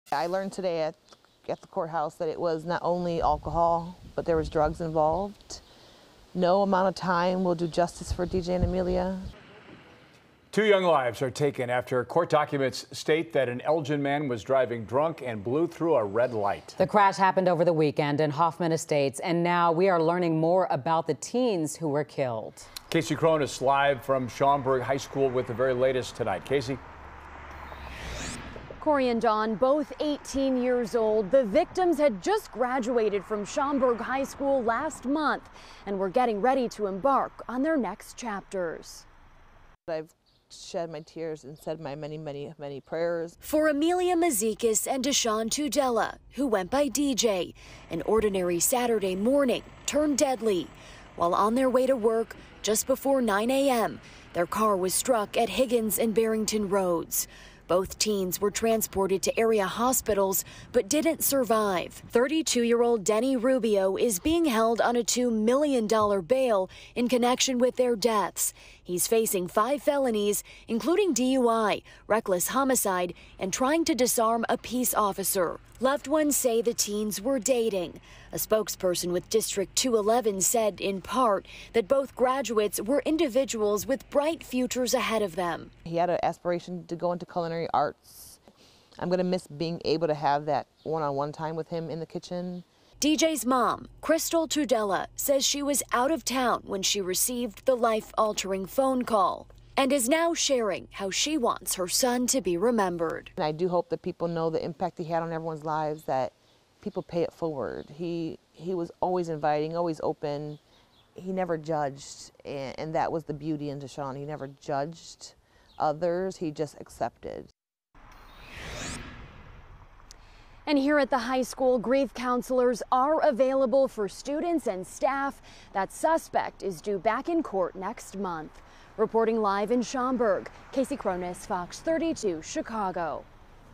Mom of suburban teen killed by alleged drunk driver speaks out
A man was arrested after two teenagers were killed in a multi-vehicle crash in Cook County Saturday morning.